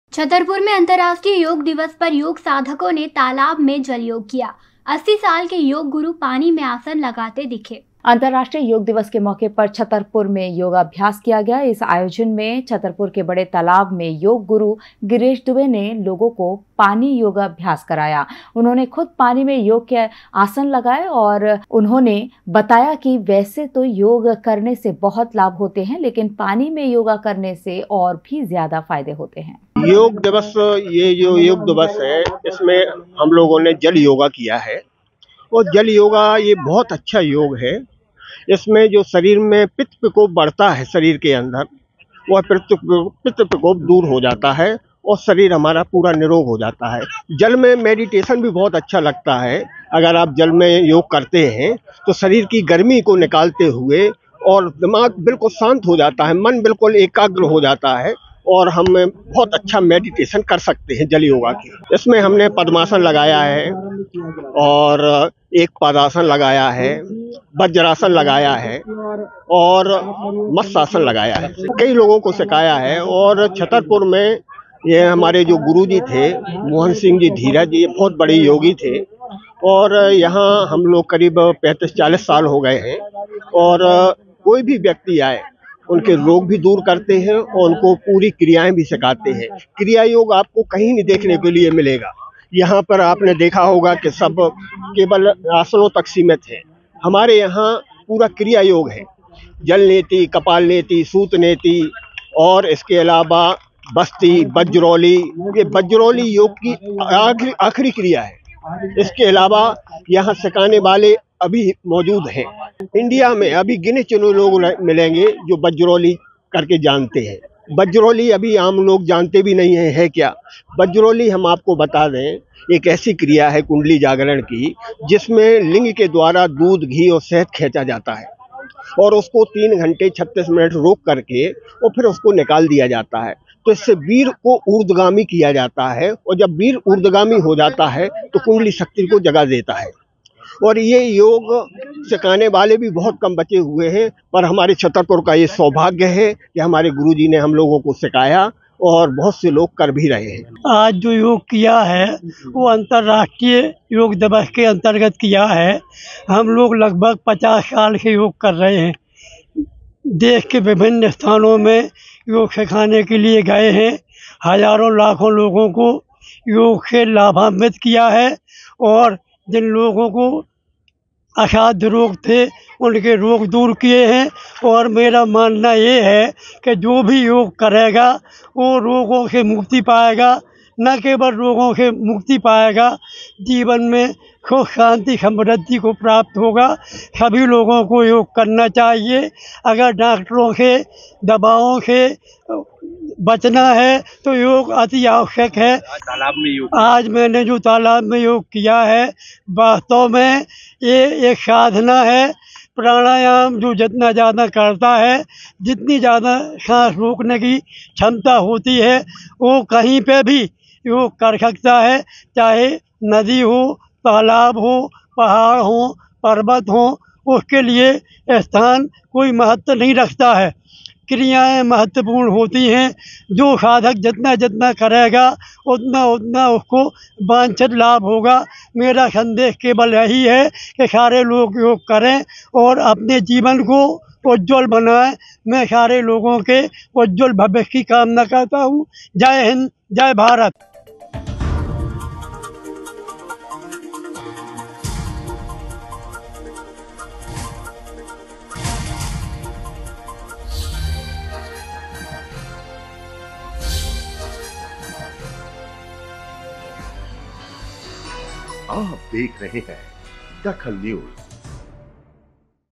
DAKHAL NEWS पानी में किया योगाभ्यास
पानी में किया योगाभ्यास
योग गुरू ने लगवाए आसन